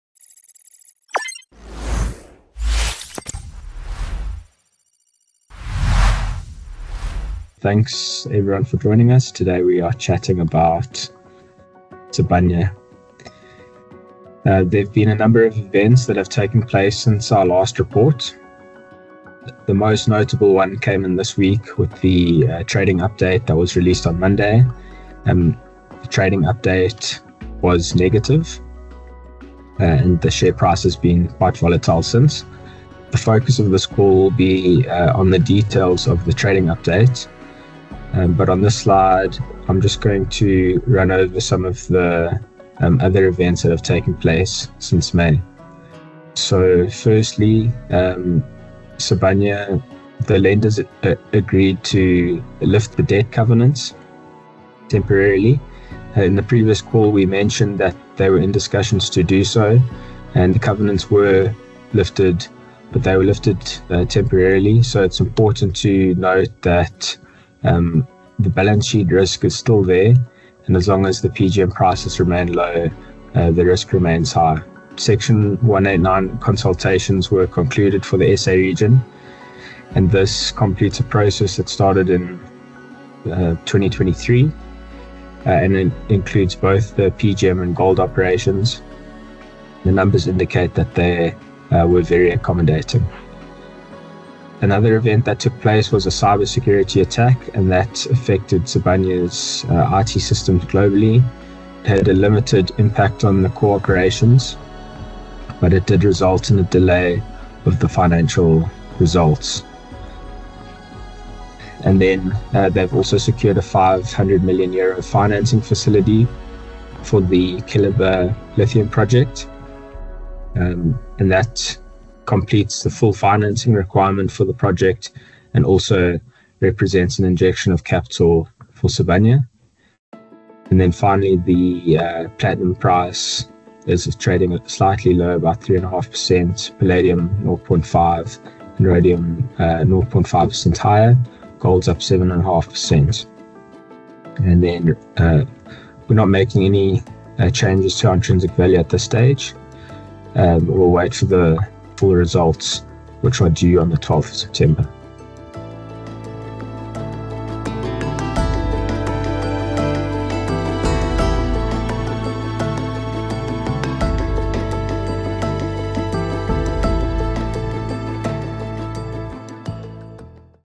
Investment idea: Sibanye Stillwater
Our last report in May covered the 1Q24 sales and operating update for Sibanye.

We highlighted balance sheet concerns and noted that the group was engaging with lenders to increase debt covenants. The following events have taken place since:

- Sibanye released a trading statement on 2 September 2024 indicating that EPS were expected to decline by more than 100% and HEPS expected to decline between 97% and 98%.
- Sibanye’s lenders have agreed to raise the net debt to adjusted EITDA debt covenant from 2.5x.
- Sibanye concluded Section 189 consultations for its SA region.